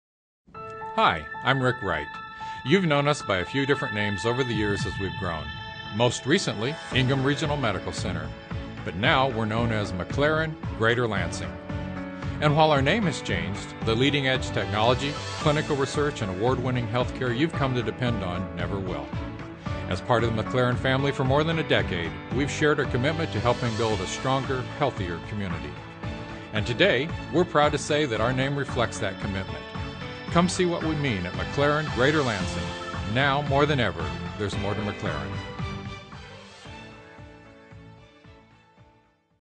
McLaren Greater Lansing: Our Name Has Changed
A word from President & CEO, Rick Wright, regarding our name change from Ingham Regional Medical Center to McLaren Greater Lansing.